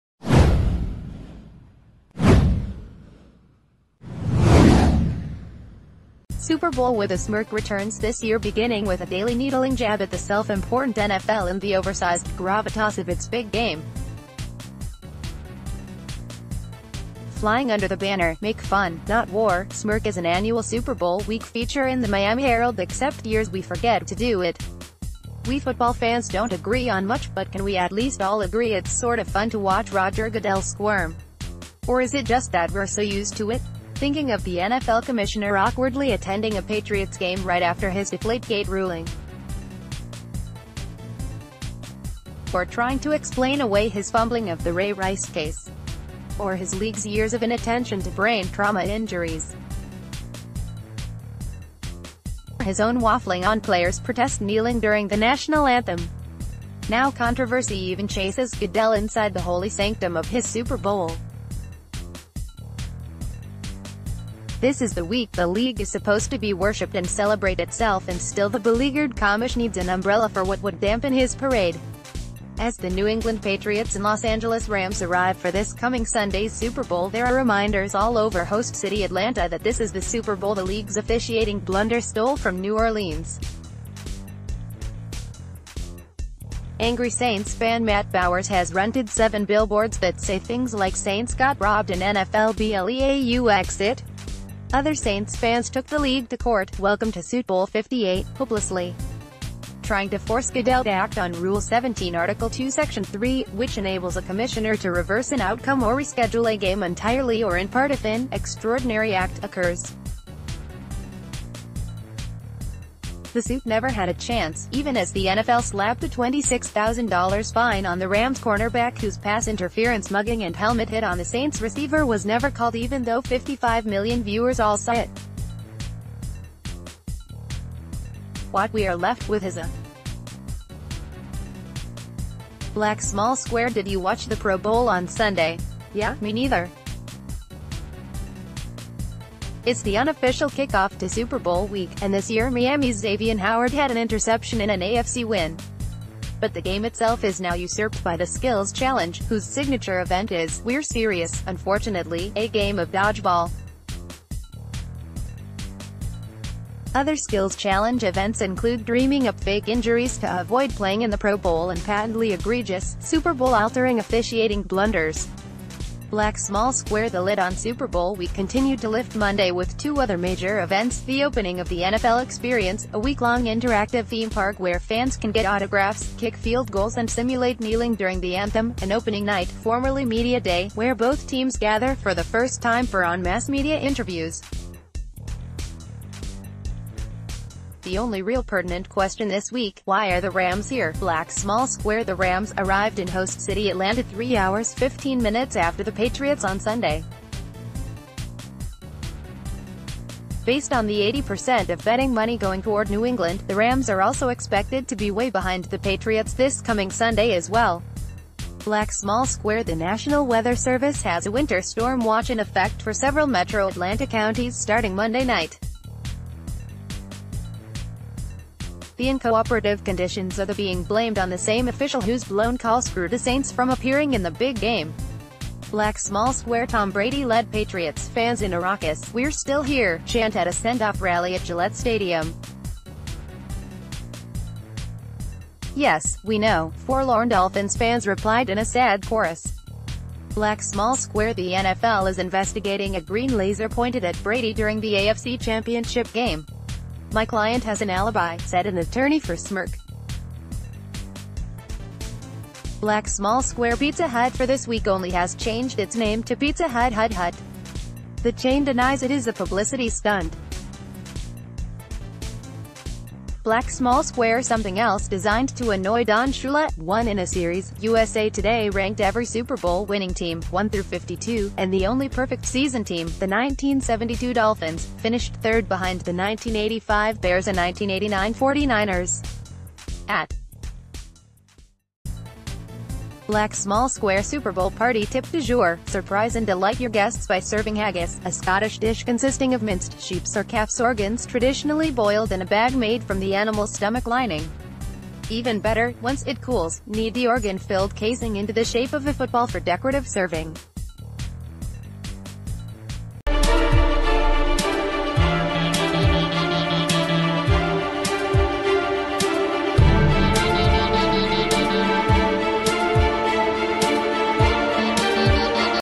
Welcome Patriots, Rams and a sweating, squirming NFL commissioner
Super Bowl With a Smirk returns this year beginning with a daily needling jab at the self-important NFL and the oversized gravitas of its big game. Flying under the banner, “Make Fun, Not War,” Smirk is an annual Super Bowl Week feature in the Miami Herald except years we forget to do it. We football fans don’t agree on much, but can we at least all agree it’s sort of fun to watch Roger Goodell squirm? Or is it just that we’re so used to it? Thinking of the NFL commissioner awkwardly attending a...